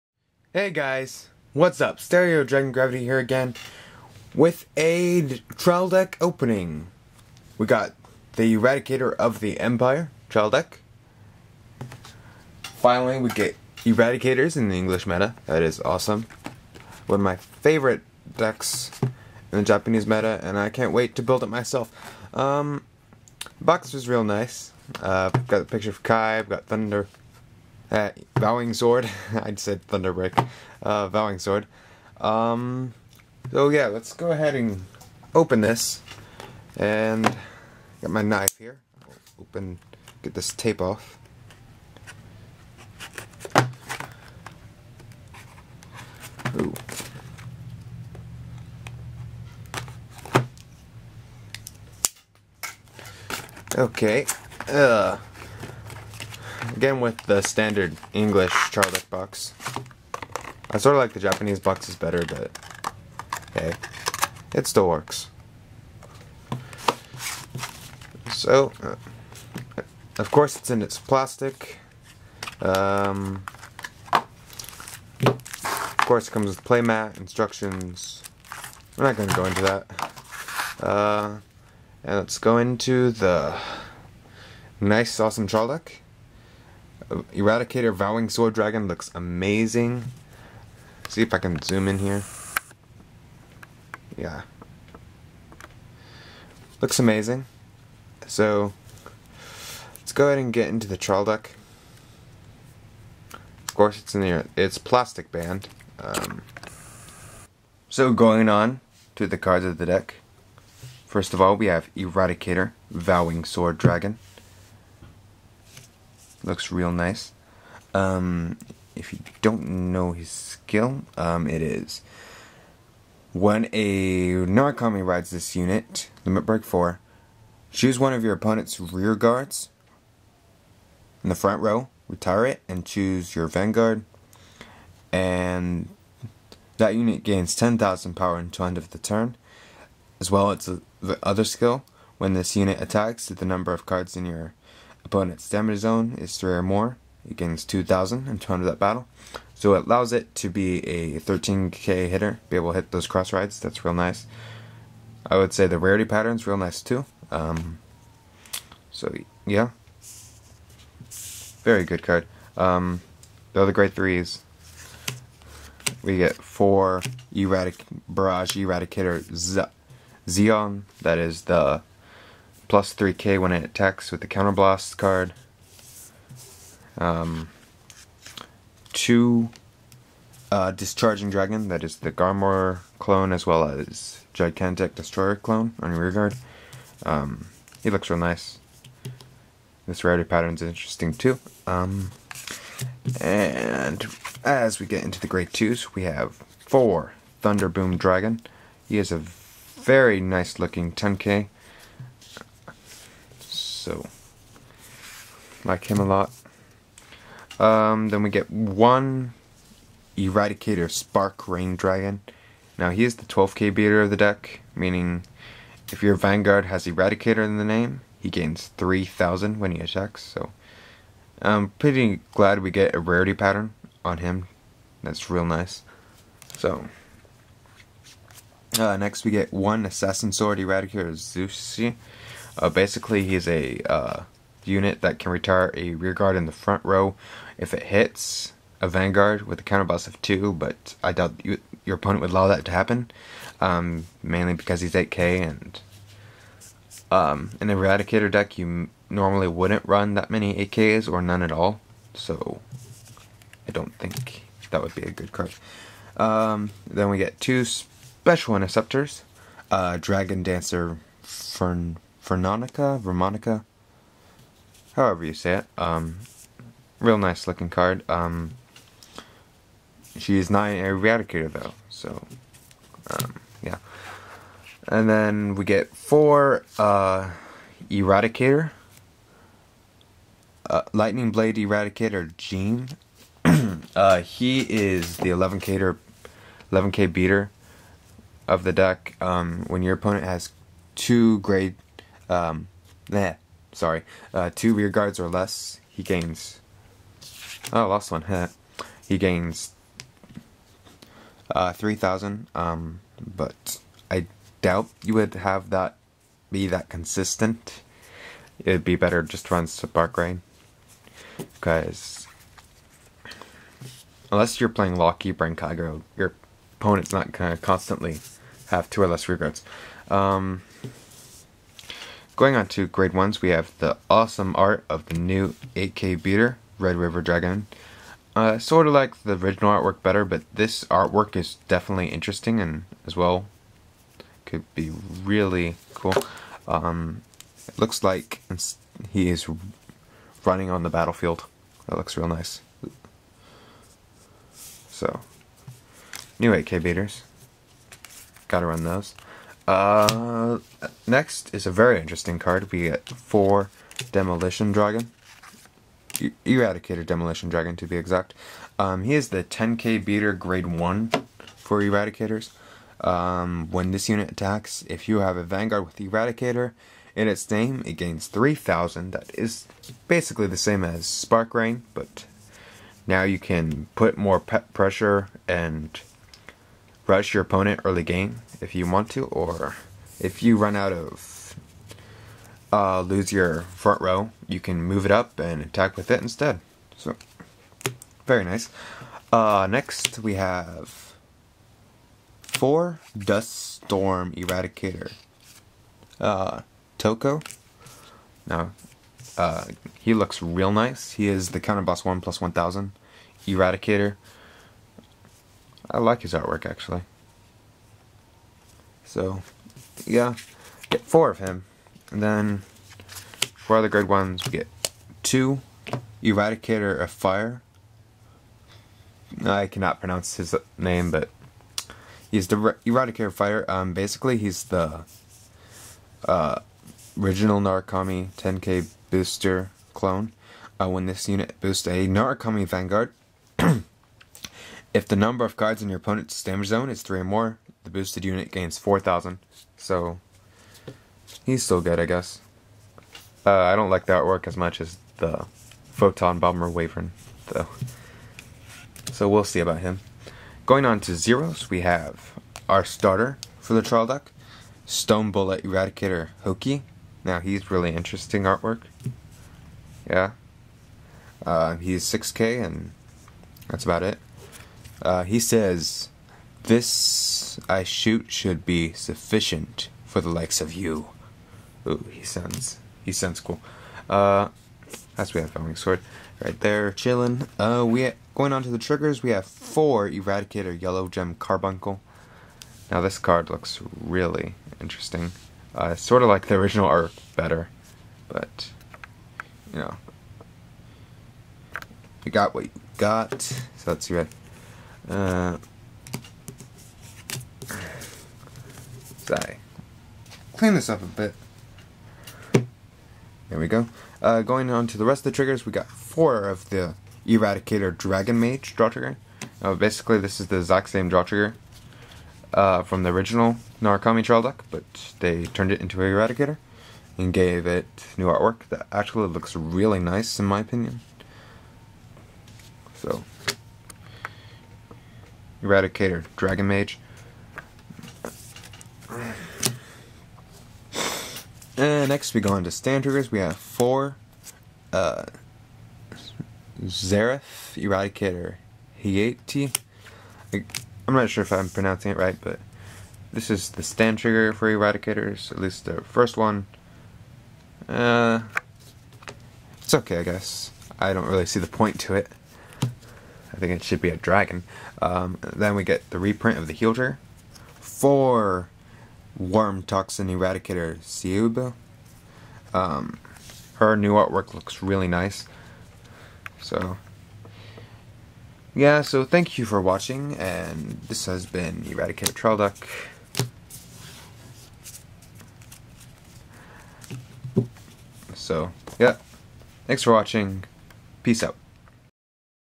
ERADICATOR OF THE EMPIRE Unboxing! | Cardfight Vanguard
Cardfight!! Vanguard Trial Deck Eradicator of the Empire Opening!

Till next flic keep Striding to Victory!
-StereoDragonGravity